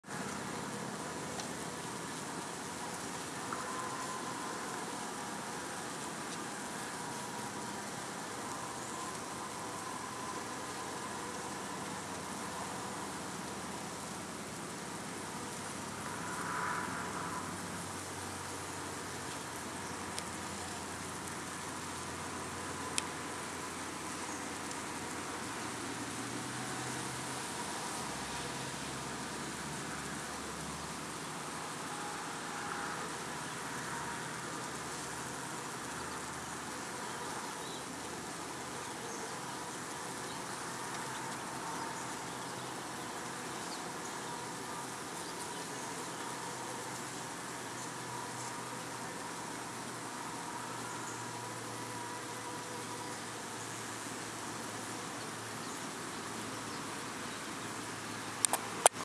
Our Italian Bees - Washboarding & Coming in for the Eve
The  mysterious honey bee group-behavior known as "washboarding" or "rocking" continues to elude an explanation, but it's fascinating to watch! Workers gather in large groups, usually pointing downward, and rock back and forth, seemingly to clean the surface beneath them. The motion has been likened to that of scrubbing clothes on a washboard.